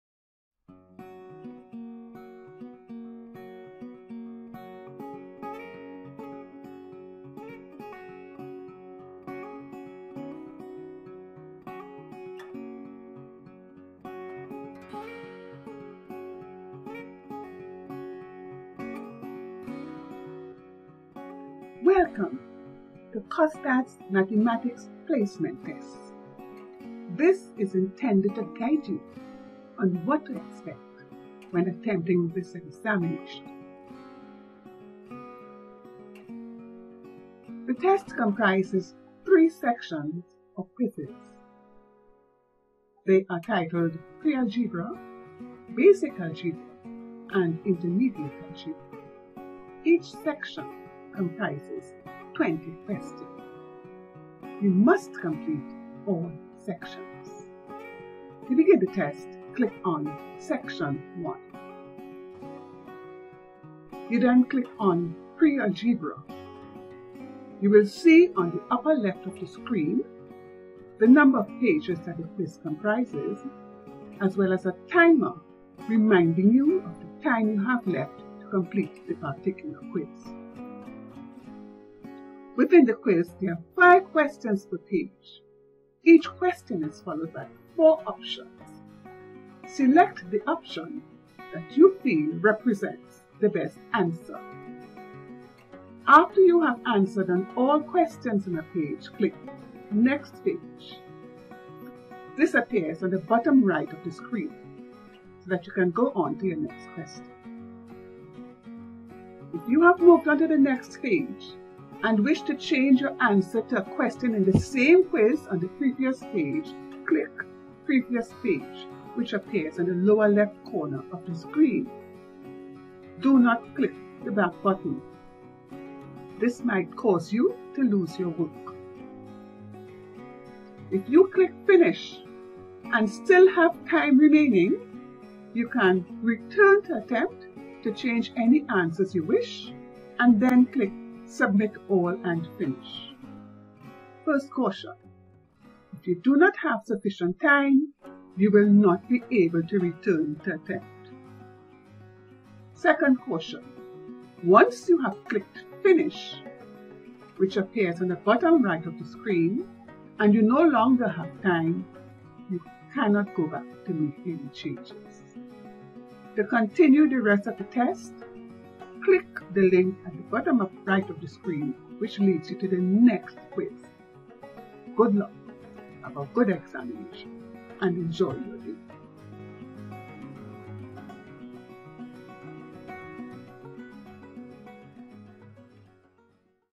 COSTAATT - Mathematics Placement Test Guide
This video will provide information on how to take the placement test